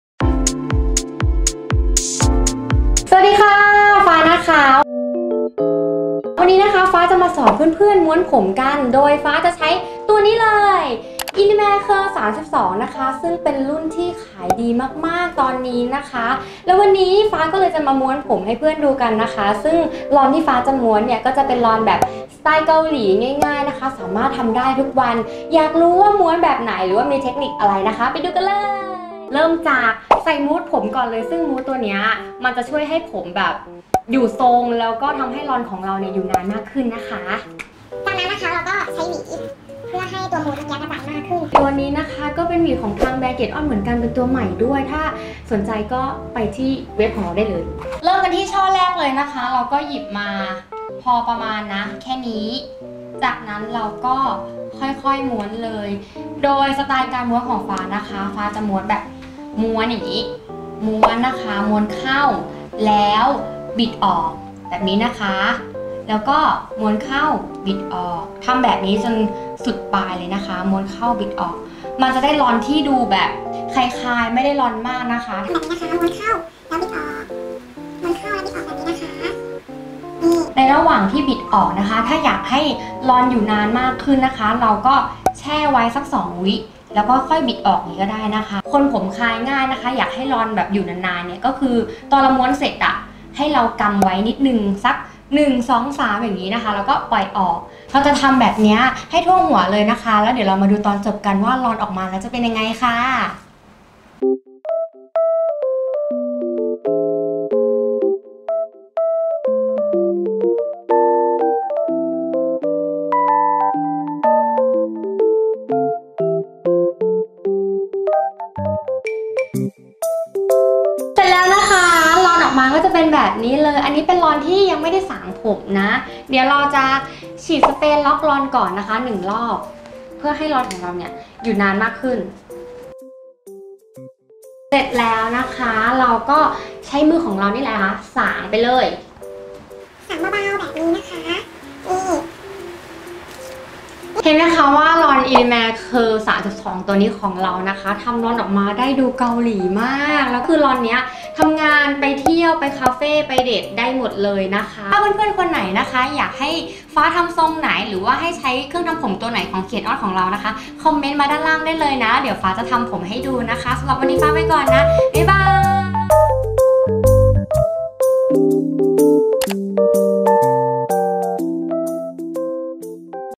Tips & Tricks ✨ การม้วนผมลอนด้วย Elemea Curl 32mm ทำง่ายๆ ได้ทุกวัน 😘
Tips & Tricks การม้วนผมด้วย Elemea Curl 32mm แบบที่เอาไปทำกันเองได้ที่บ้าน แต่สวยเหมือนออกจากร้าน